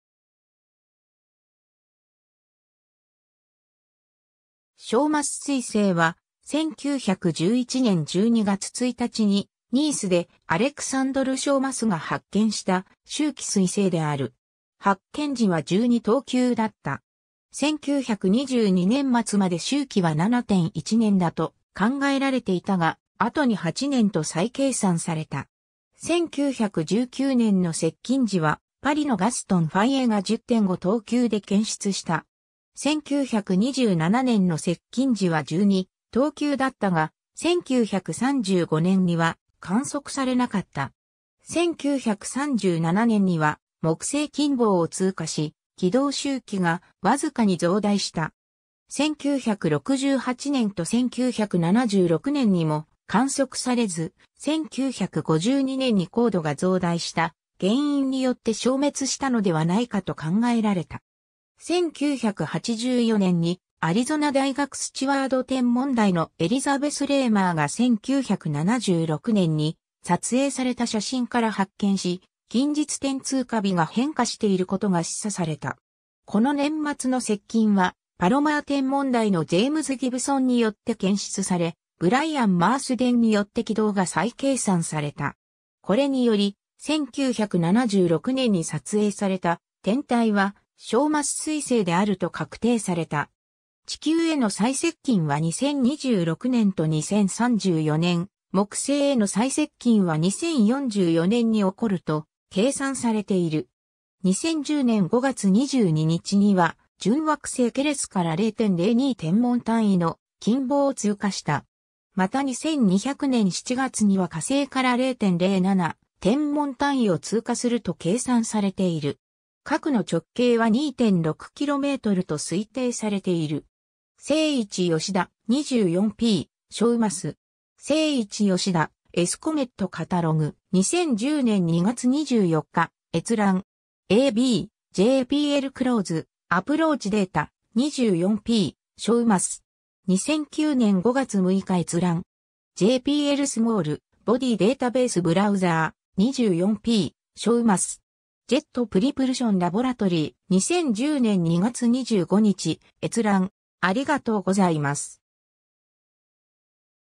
ショーマス彗星
ショーマス彗星(24P/Schaumasse)は、1911年12月1日にニースでアレクサンドル・ショーマスが発見した周期彗星である。
発見時は12等級だった。
 1922年末まで周期は7.1年だと考えられていたが、後に8年と再計算された。
1919年の接近時は、パリのガストン・ファイエが10.5等級で検出した。
 1927年の接近時は12等級だったが、1935年には観測されなかった。
1937年には木星近傍を通過し、軌道周期がわずかに増大した。
 1968年と1976年にも観測されず、1952年に光度が増大した原因によって消滅したのではないかと考えられた。
1984年にアリゾナ大学スチュワード天文台のエリザベス・レーマーが1976年に撮影された写真から発見し、近日点通過日が変化していることが示唆された。
この年末の接近はパロマー天文台のジェームズ・ギブソンによって検出され、ブライアン・マースデンによって軌道が再計算された。
これにより、1976年に撮影された天体はショーマス彗星であると確定された。
 地球への最接近は2026年と2034年、木星への最接近は2044年に起こると計算されている。
 2010年5月22日には、準惑星ケレスから0.02天文単位の近傍を通過した。
また2200年7月には火星から0.07天文単位を通過すると計算されている。
 核の直径は2.6kmと推定されている。
 ^ Seiichi Yoshida (2009年4月7日). “24P/Schaumasse”. Seiichi Yoshida's Comet Catalog. 2010年2月24日閲覧。
 ^ a b “JPL Close-Approach Data: 24P/Schaumasse” (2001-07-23 last obs). 2009年5月6日閲覧。
 ^ “JPL Small-Body Database Browser: 24P/Schaumasse”. Jet Propulsion Laboratory (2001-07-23 last obs). 2010年2月25日閲覧。